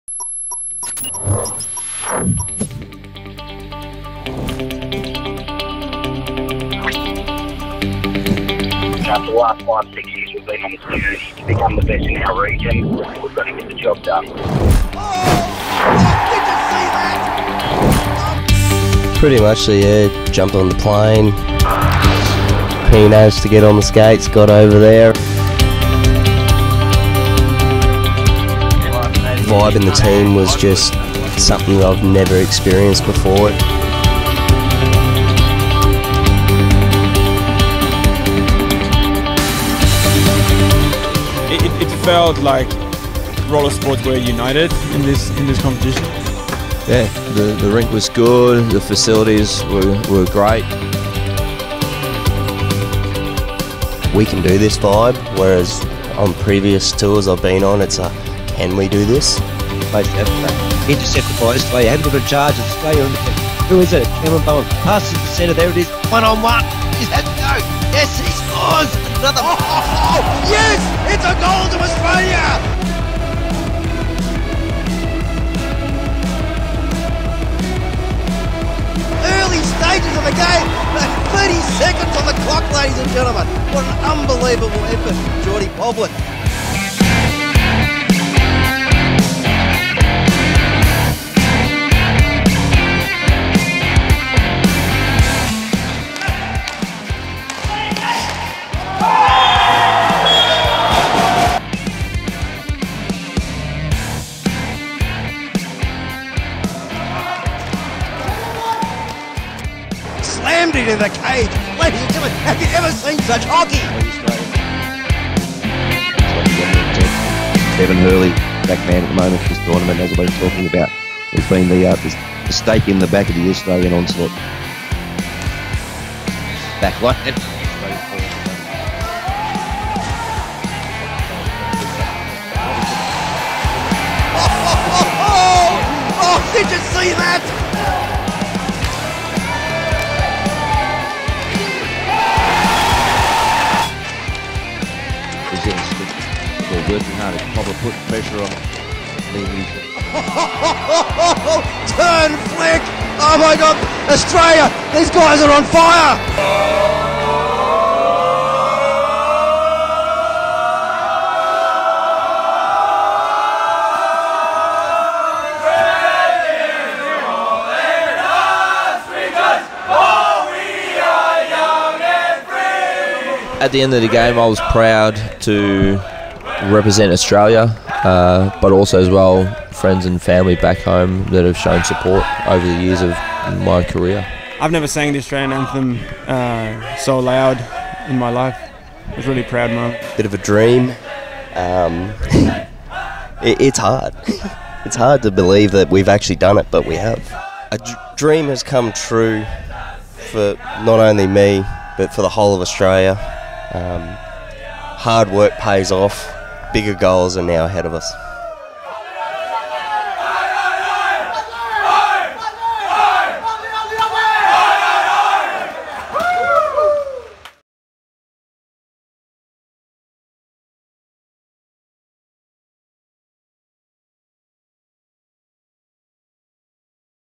Asia Oceania Roller Hockey Championships - Highlights
A brief doco' styled highlights reel of the Australian Roller Hockey Teams gold medal win in South Korea 2018.
Drew Media Media is the Official Media Partner for Roller Hockey Australia.